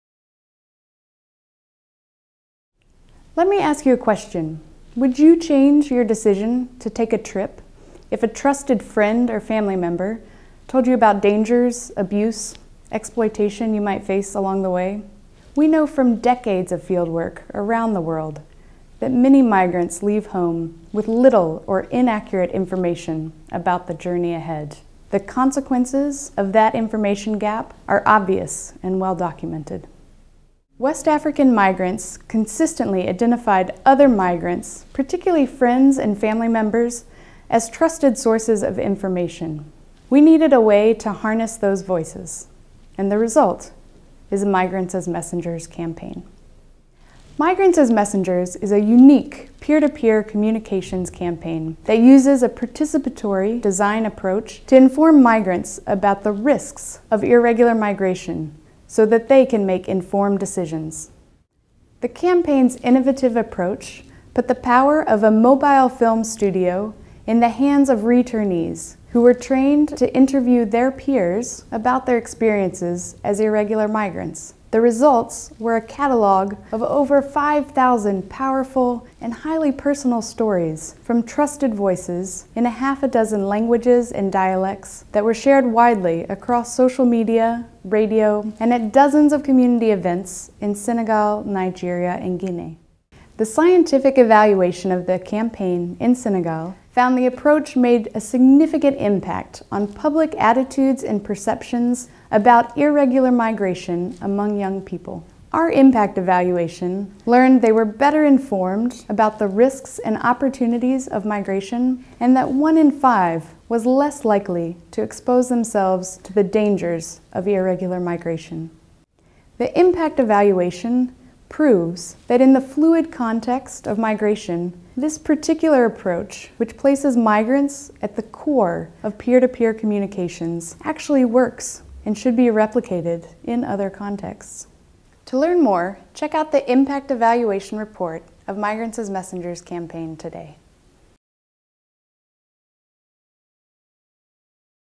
Irregular Migrants are often exposed to danger, abuse and exploitation
How can we raise awareness of those risks and offer them the ability to make safe migration decisions? Our new-impact evaluation study assesses the effects of peer-to-peer campaigns on potential migrants in West Africa.